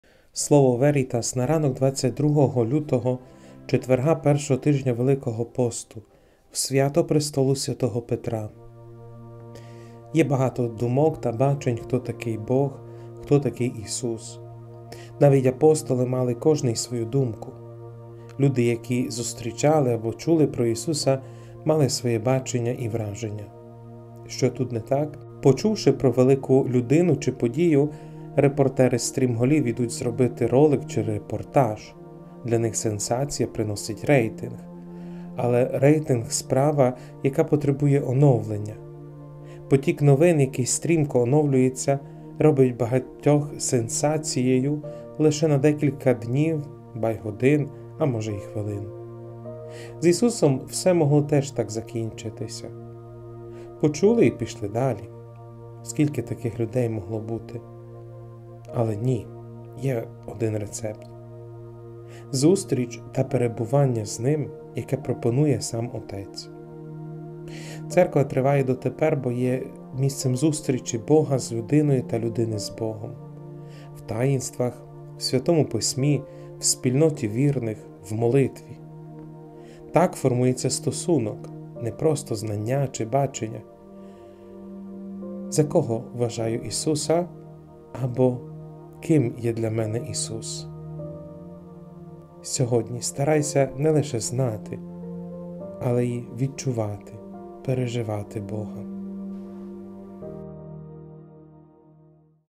Слово Veritas на ранок . 22 лютого, середа І тижня Великого Посту
Слово Veritas на ранок . 🙌 Почни день з Богом!
🔖 22  лютого,  середа  І тижня Великого Посту
Хто такий Бог? Хто такий Ісус?
Сенсація приносить рейтинг. Рейтинг тимчасовий.
Ким є для мене Ісус? Не тільки знайти, але й переживати. Відчувати Бога

Роздуми: о. Сергій Герзанич

📖ПЕРШЕ ЧИТАННЯ 1 Пт 5, 1-4
Читання з Першого послання святого апостола Петра.
Улюблені! Пресвітерів, які  є між вами, благаю я, співпресвітер і свідок Христових страждань, співучасник слави, що має відкритися: пасіть те Боже стадо, що є у вас; наглядайте не з примусу, а добровільно, по-Божому; не задля ненаситної жадоби користі, а з ревності; не як ті, хто панує над спадком, але як ті, хто сам є прикладом для стада; і коли з’явиться Архіпастир, одержите нев’янучий вінець слави.  Слово Боже.

🕊ЄВАНГЕЛІЄ Мт 16, 13-19
† Читання святого Євангелія від Матея.
Того часу, прийшовши в околиці Кесарії Филипової, Ісус звернувся до своїх учнів, запитуючи: «За кого люди вважають Сина Людського?» Вони відповіли: «Одні  – за Йоана Хрестителя, другі – за Іллю, інші – за Єремію або за одного з пророків». Каже їм Ісуc: «А ви Мене за кого вважаєте?» У відповідь Симон-Петро промовив: «Ти є Христос, Син Бога живого!» А Ісус відповів йому і сказав: «Блаженний ти, Симоне, сину Йони, бо не тіло і кров тобі  це відкрили, а Мій Отець Небесний. І Я тобі  кажу, що ти є Петро – скеля; і на цій скелі Я збудую свою Церкву, і брами аду не здолають її. Я дам тобі  ключі  Царства Небесного: і те, що ти зв’яжеш на землі, буде зв’язане на небесах; і те, що ти розв’яжеш на землі, буде розв’язане на небесах».  Слово Господнє.